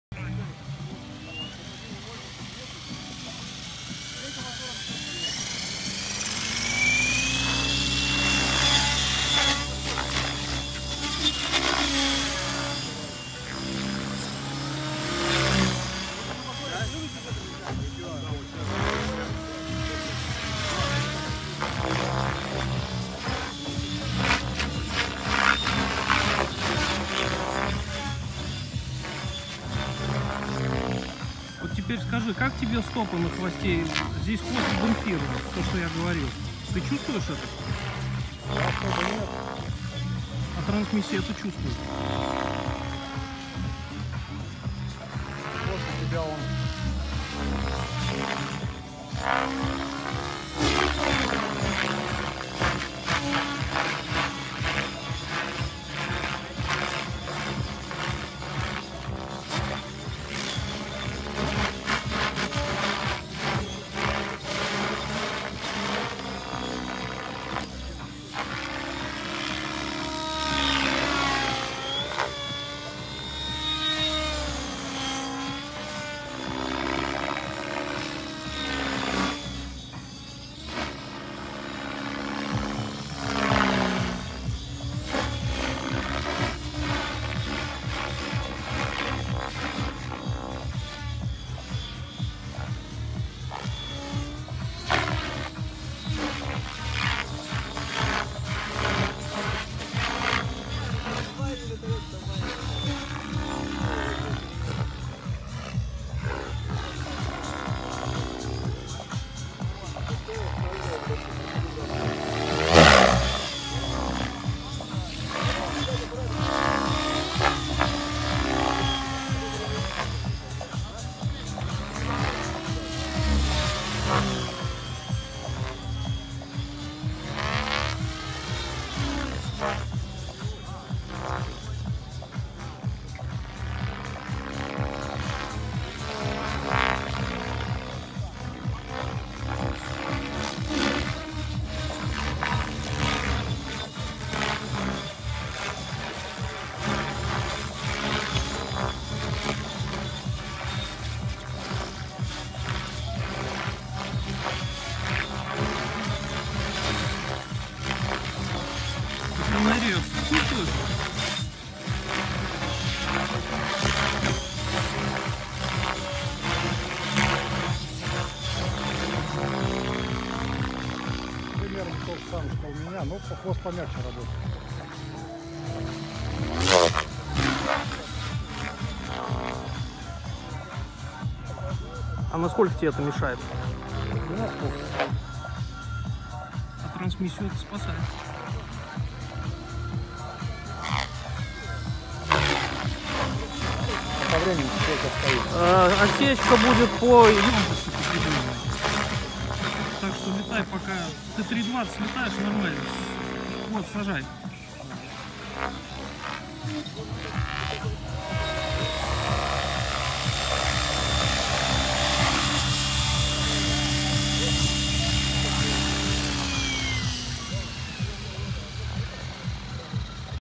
Звенигород 2018, Илья Банаев тестирует мой вертолет
Вердикт Ильи: тупой циклик. Рекомендовал проставки на серво. Также Илья отметил, что брайн в целом ему нравится больше вибара :)